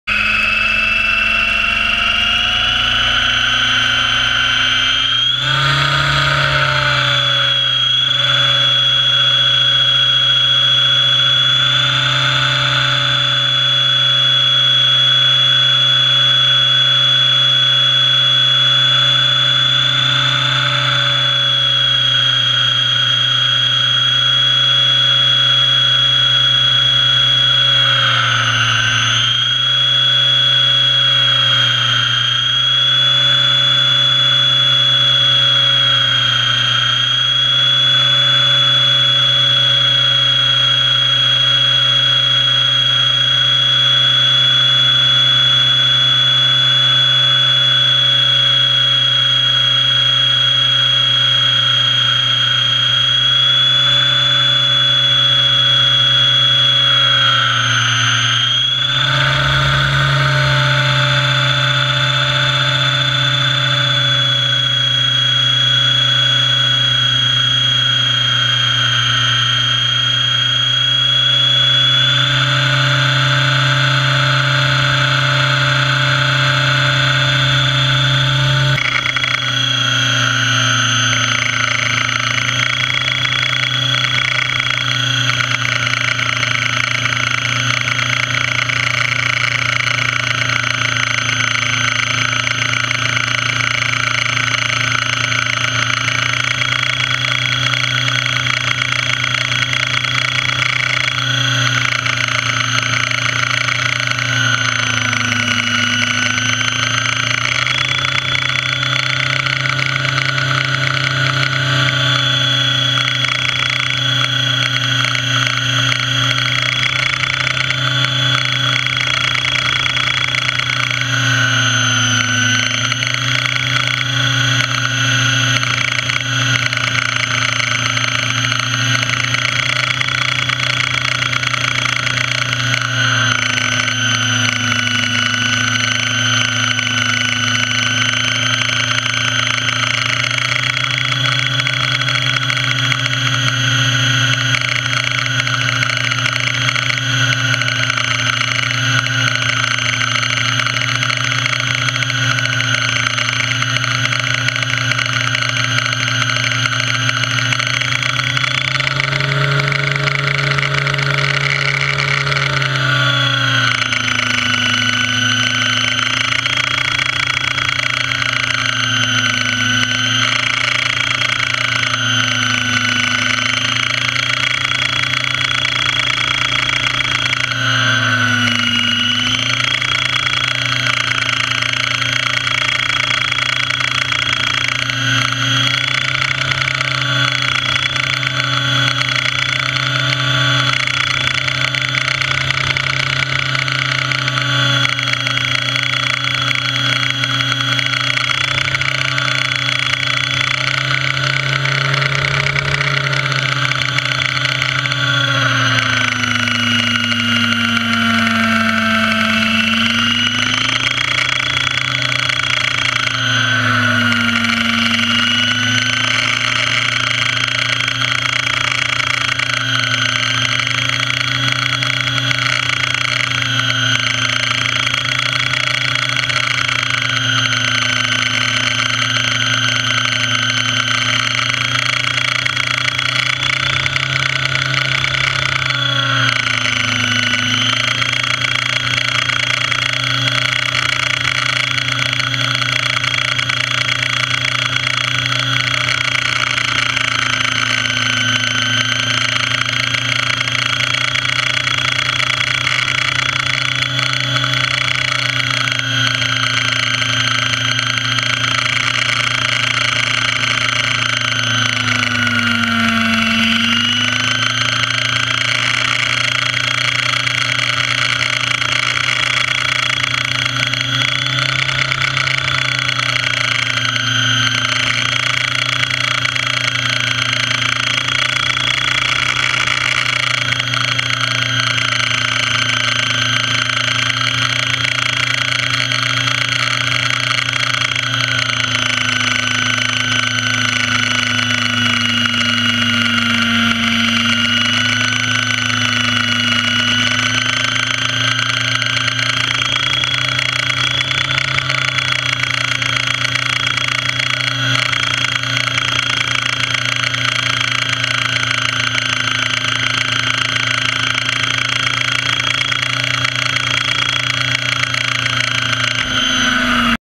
Model helicopter on Gorleston beach.
Model helicopter on Gorleston beach with footage taken from a Gopro, unfortunately the mount moved whilst flying, will try a different one next time.